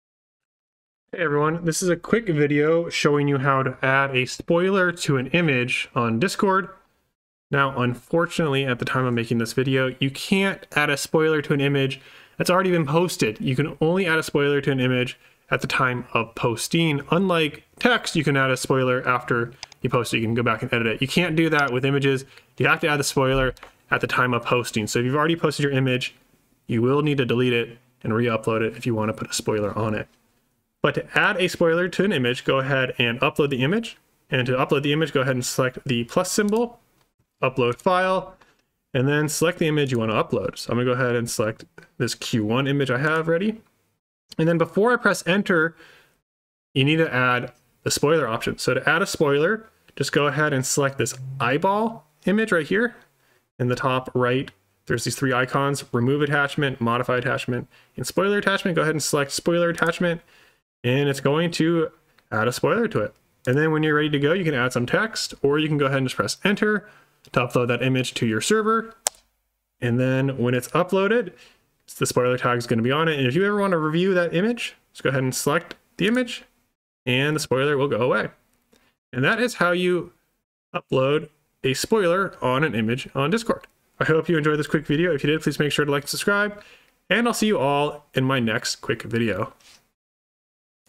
How to Upload an Image with a Spoiler on Discord
This video shows how to add a spoiler tag to an image on Discord.